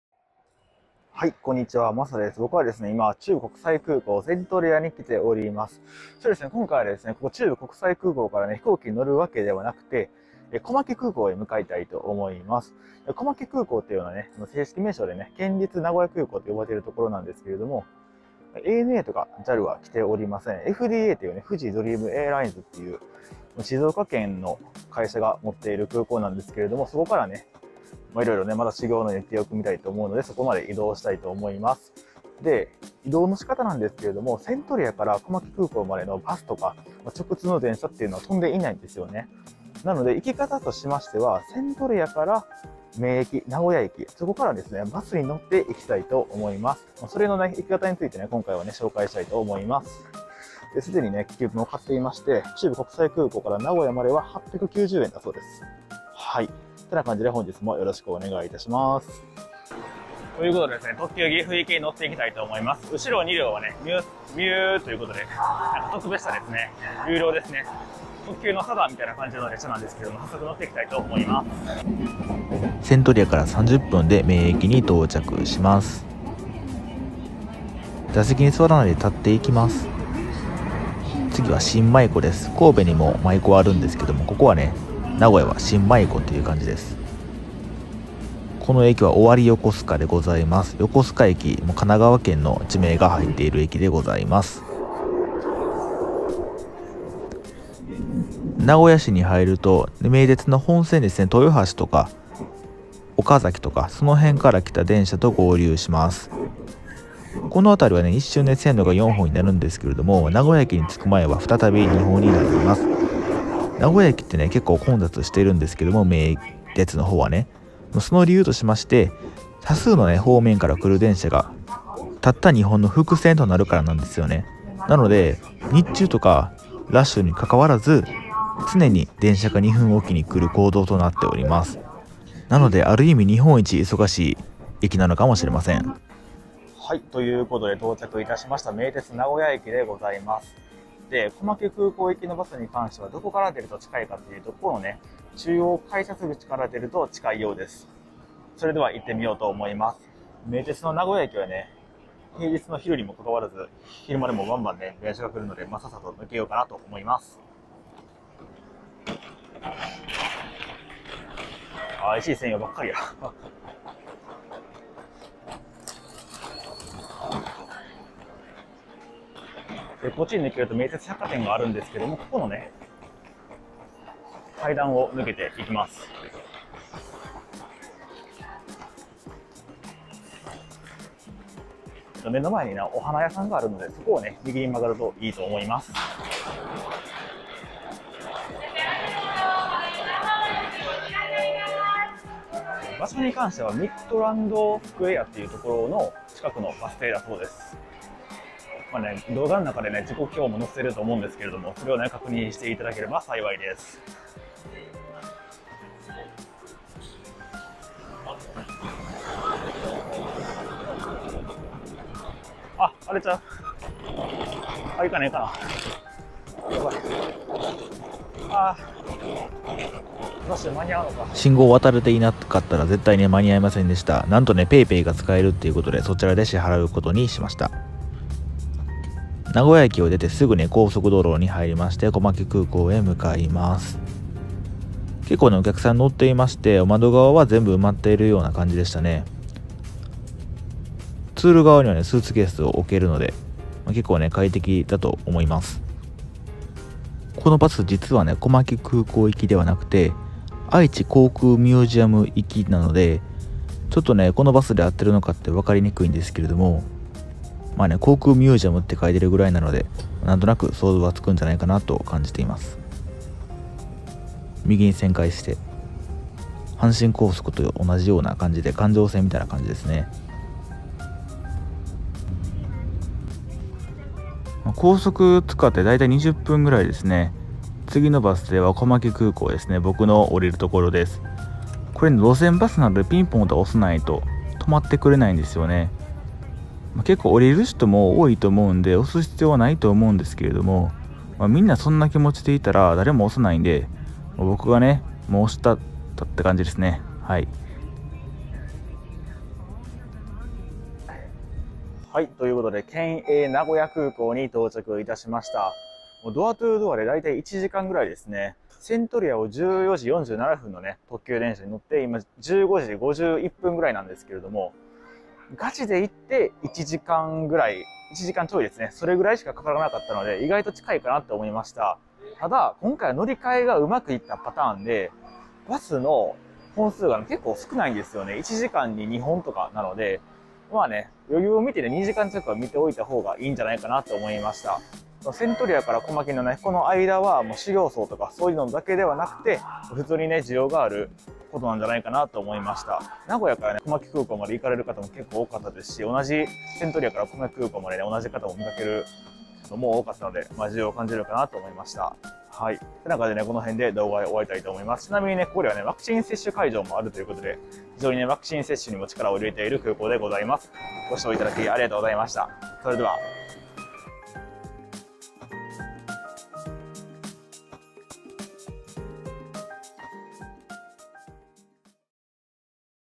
【快適】セントレアから小牧空港まで移動してみた！
小牧空港はJALやANAが飛んでおらず、FDAという会社の飛行機が飛んでいる、少しマイナーな空港です。
しかし、名古屋から青森、山形など、地方を結ぶ路線が数多く就航しているため、中京圏の人にとっては使い勝手の良い空港なのかもしれません。